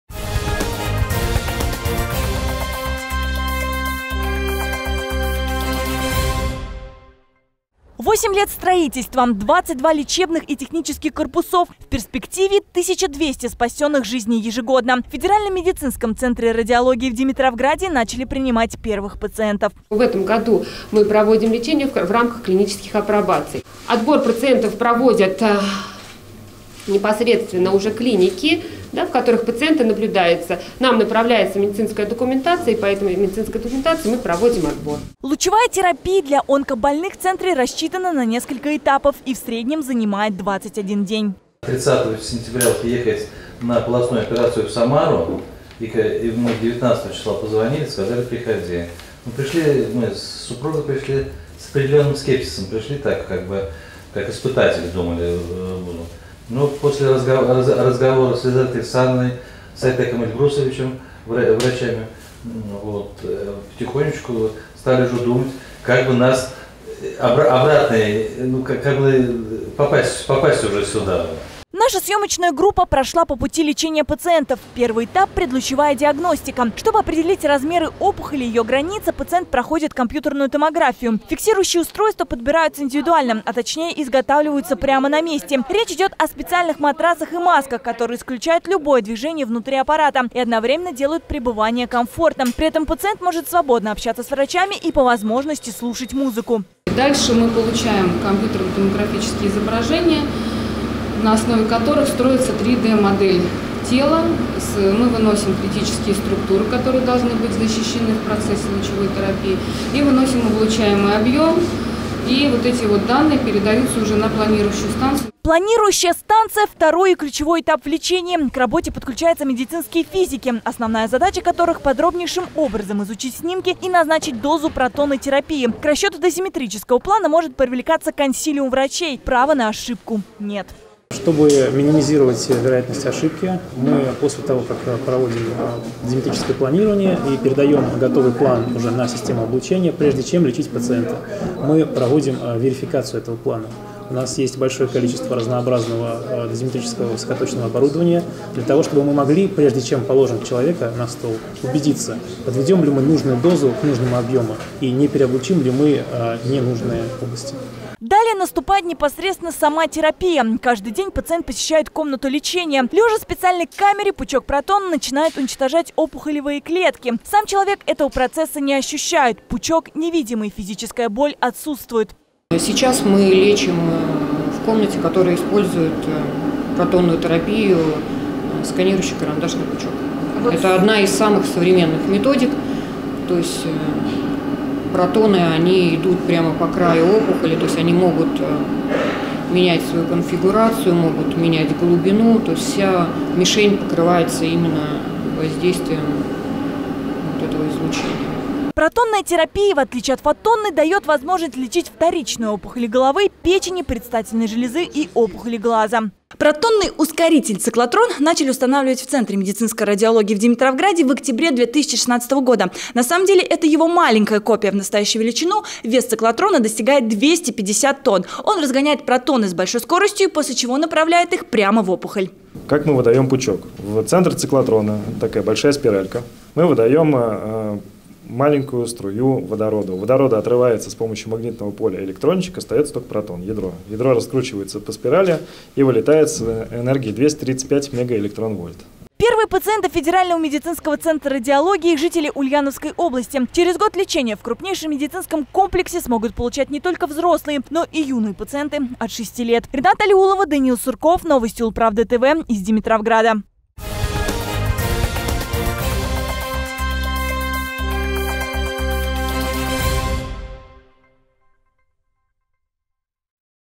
Все - пучком. Медицинский центр радиологии в Димитровграде принял первых пациентов
8 лет строительства. 22 лечебных и технических корпусов. В перспективе - 1200 спасенных жизней ежегодно. В Федеральном медицинском центре радиологии в Димитровграде начали принимать первых пациентов.